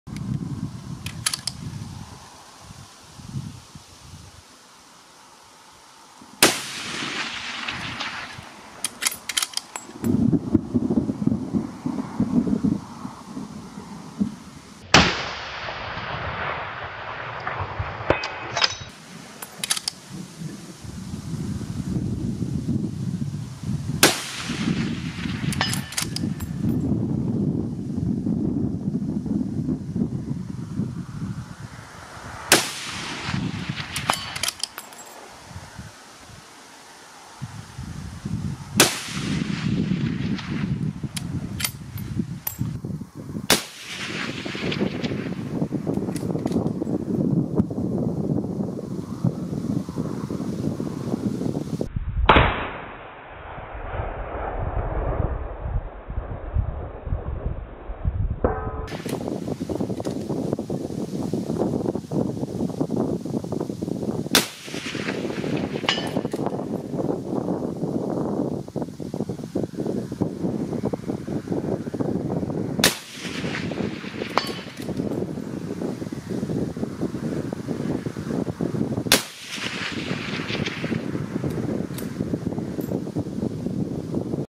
Shot is rarely taken prone. Hog saddle mount & Sirui R-5214X tripod. 338 Norma Magnum RS9 & ZCO 420
Haenel RS9 338 Norma Magnum on tripod.
Standing postion, distance 380-meters.
300gr Norma ammo, Sierra MatchKing HBPT bullets.